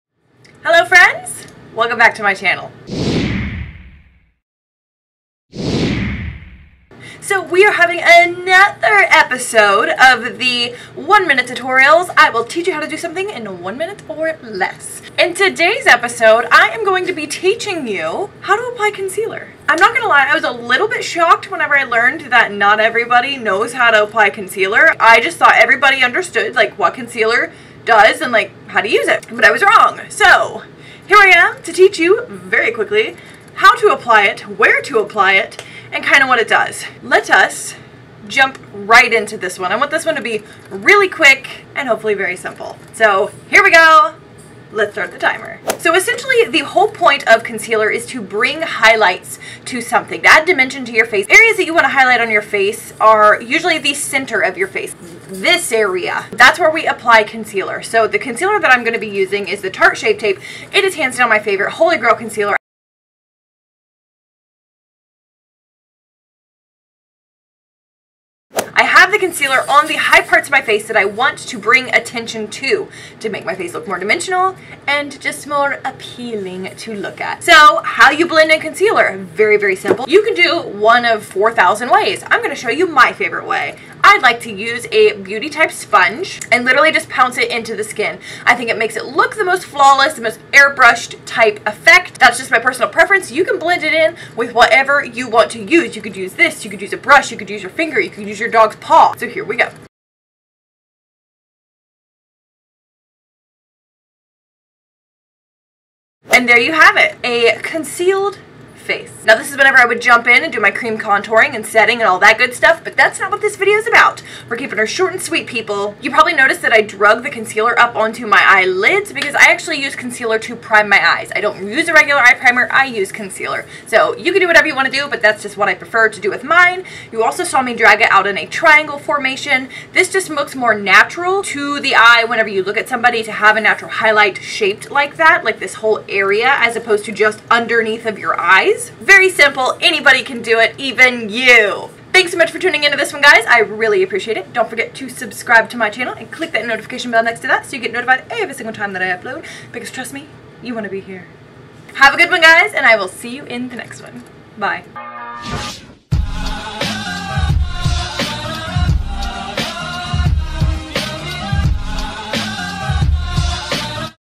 One Minute Tutorials | Concealer | MORGAN MULLINS
Hello friends! Welcome back to this week's episode of One Minute Tutorials! I think you're going to really like this one. It's all about concealer. Such a simple concept/product but I've learned so many people just don't know how to use it! So let me teach you how to apply concealer in one minute or less!

Extras!

Star burst: I attempted to give credit for this but now the link states "This video has been removed"

The end card to this video is not mine. It was made by a creator here on YouTube. The link to their video is no longer active. If you know who created this end card, let me know so I can give them credit!

Thanks for tuning in again to this one! I really appreciate it!